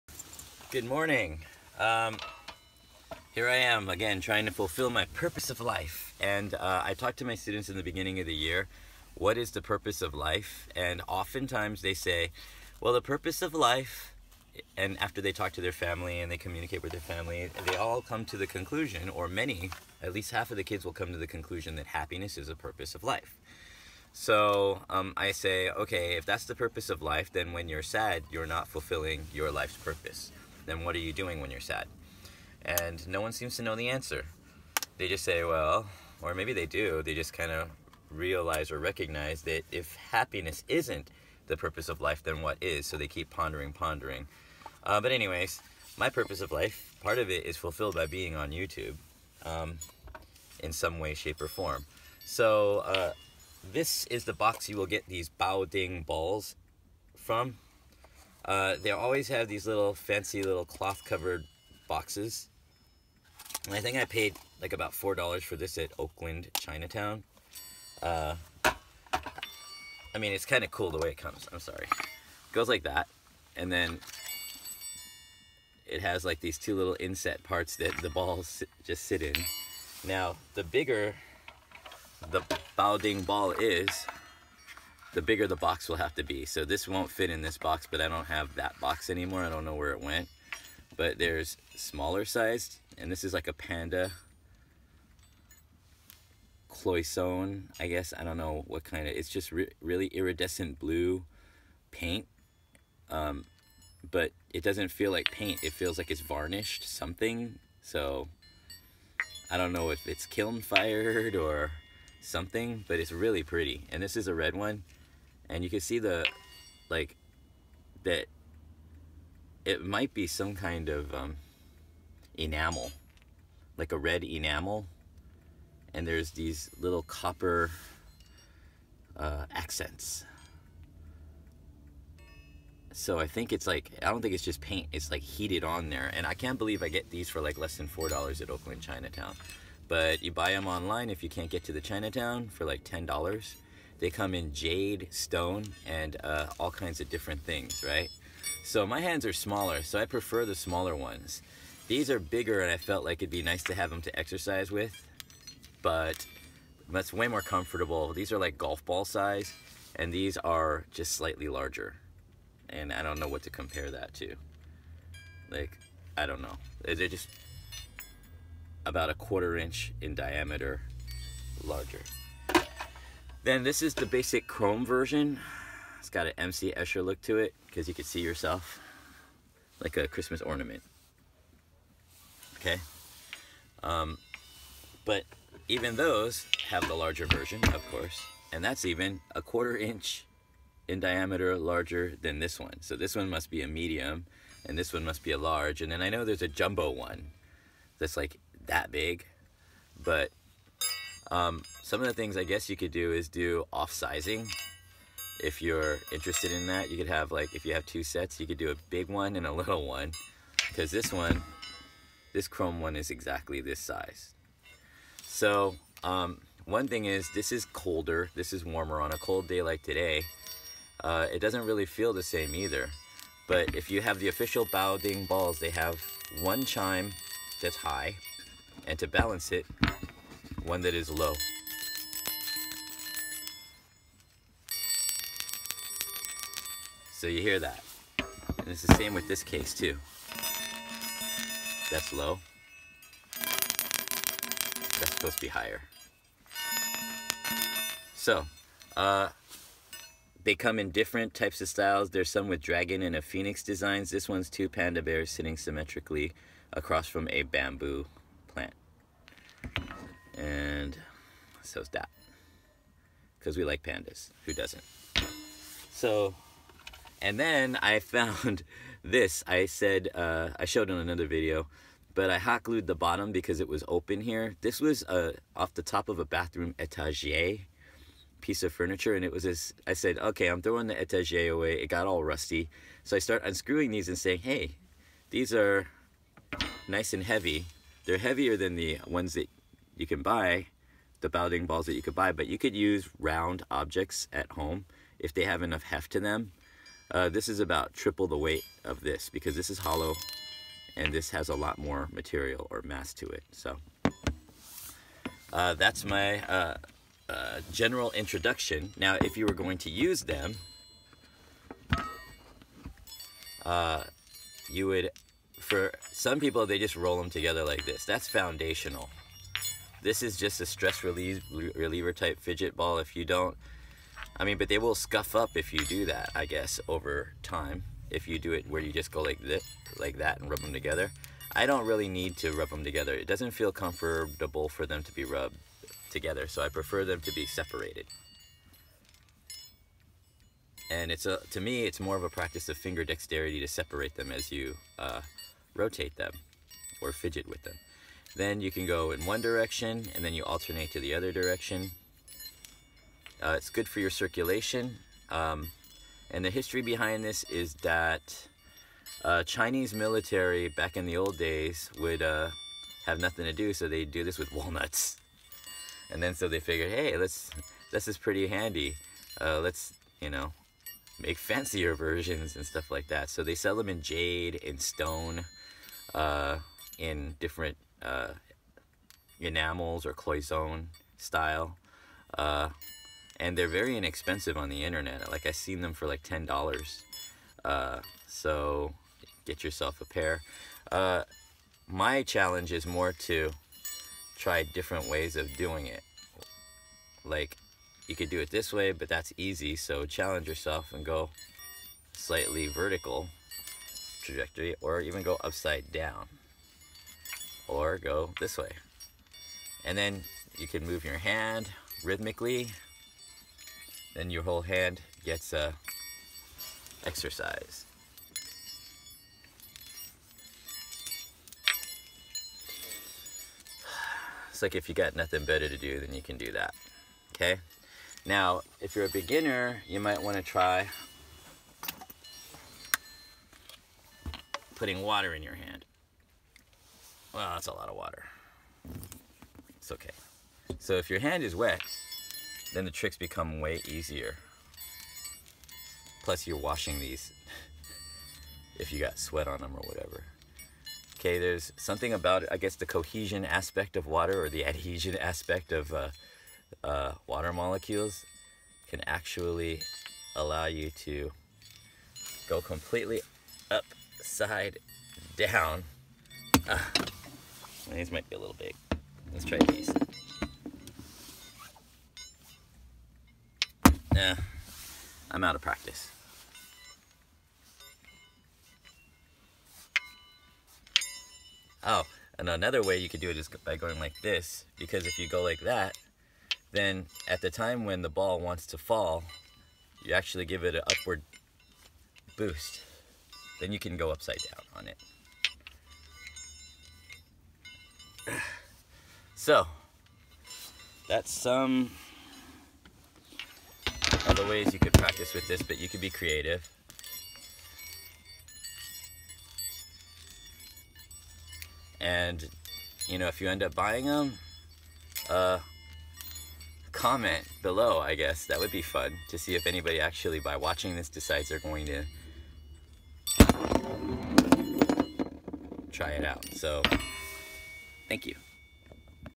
Bao Ding Ball Ancient Chinese Secret Fidget Spinner
Bao Ding balls are ancient fidget spinners from China. I learned how to use these on my own because back when I bought them there weren't any instructions. You can use them my way or figure out new ways. Get them online for about $10 a pair.